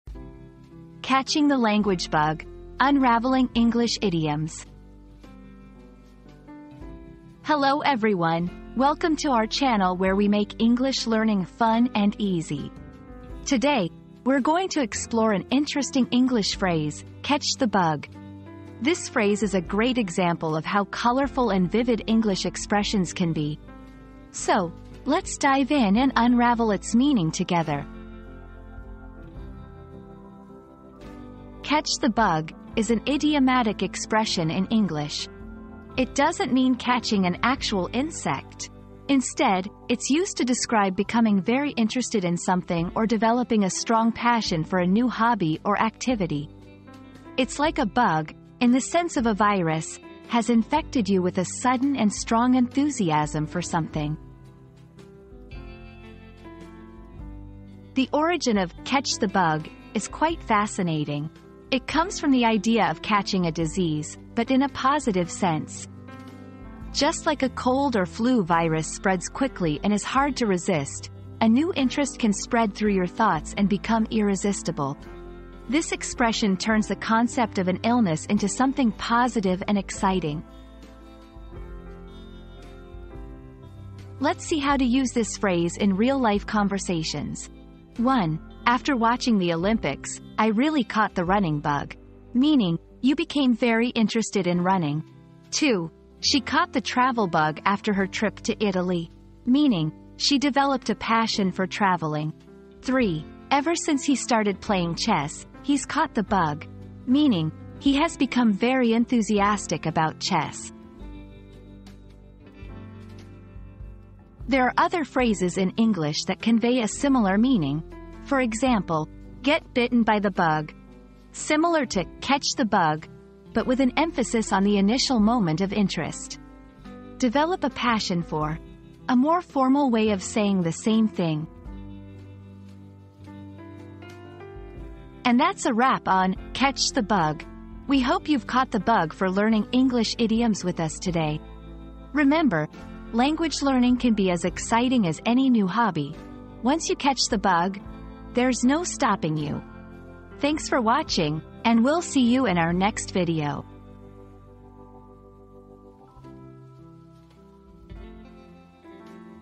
Catching the Language Bug: Unraveling English Idioms
00:00 • Introduction - Catching the Language Bug: Unraveling English Idioms
00:31 • What Does "Catch the Bug" Mean?
00:57 • Origins of the Phrase
01:23 • Using "Catch the Bug" in Sentences
01:53 • Similar Expressions